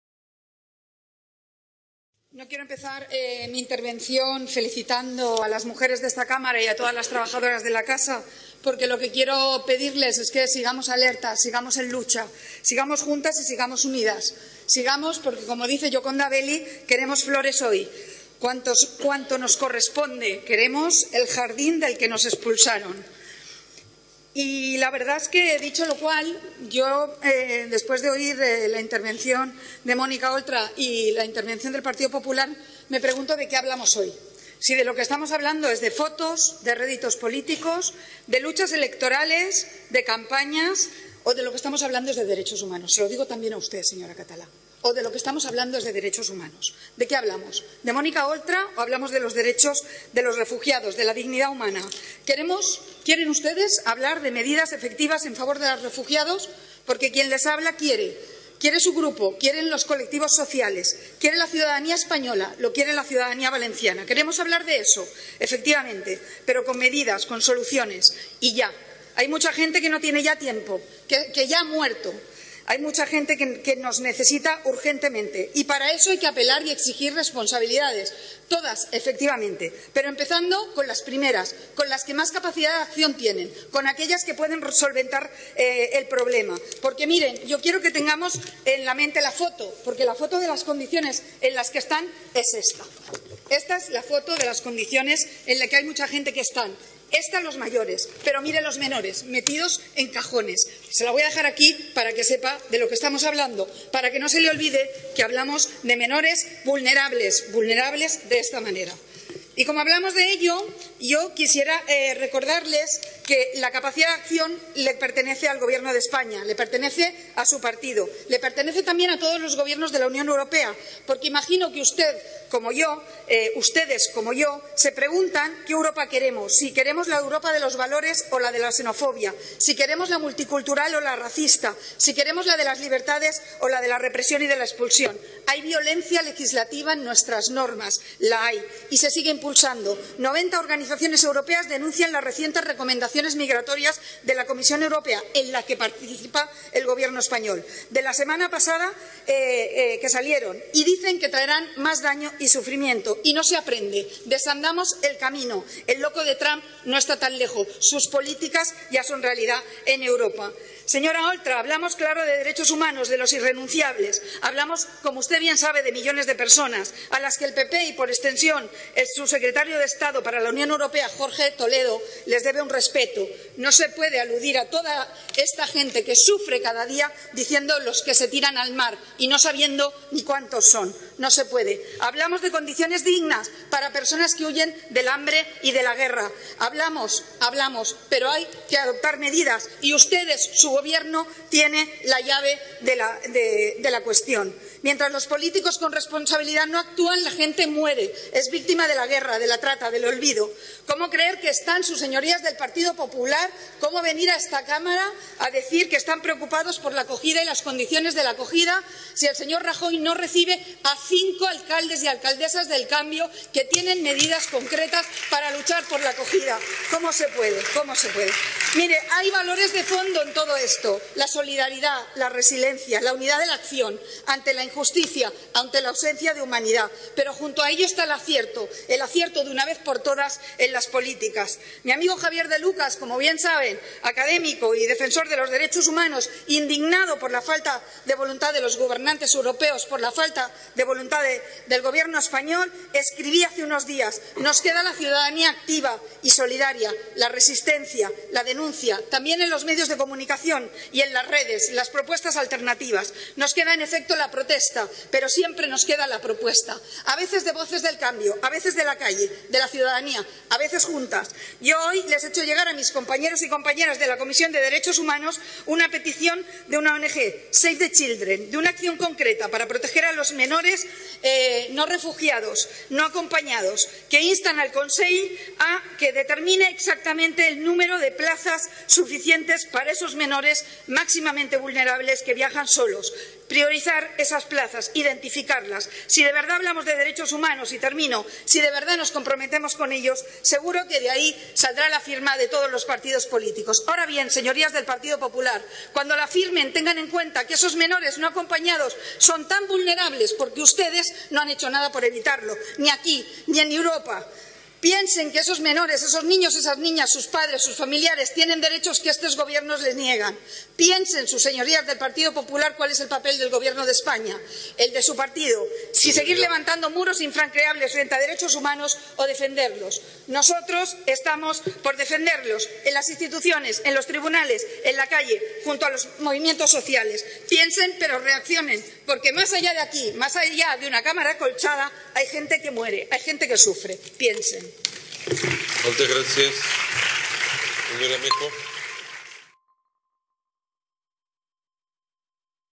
PleCorts Intervención de Fabiola Meco sobre los Refugiados 8/3/2017
Posició de Podem respecte a la compareixença de la vicepresidenta del Consell i consellera d’Igualtat i Polítiques Inclusives per a explicar quins mitjans ha ofert la Generalitat al govern de la nació per a acollir als refugiats sirians que arribena Espanya, sol·licitada pel Grup Parlamentari Popular